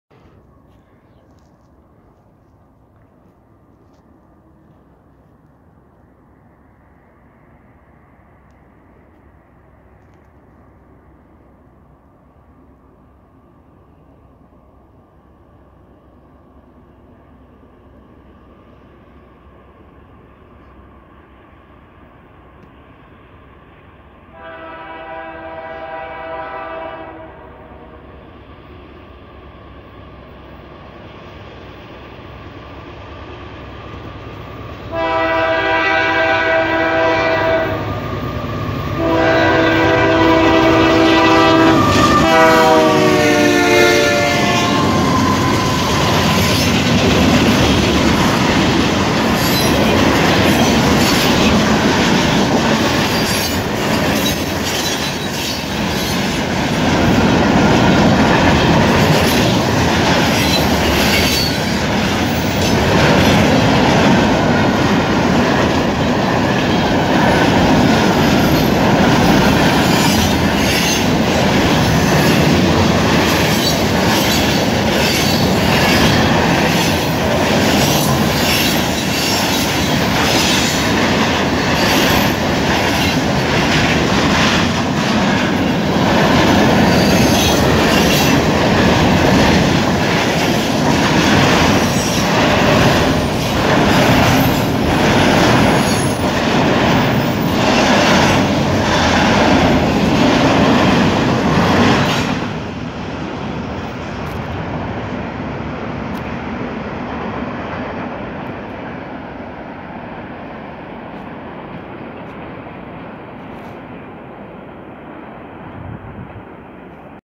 BIG RED GEVOS! CPR 9622 AT PENDER (DEC 31/18)
CPR 9622, CPR 9803, CPR 9750 (CPR 240) (60 cars) eastbound around the curve at Pender just west of my usual spot.
If you guys like this angle, I might start do more railfanning at this crossing. If not, the other crossing is still available.

MP 81.3 CPR Galt sub (Pender)
Innerkip, Ontario
1242h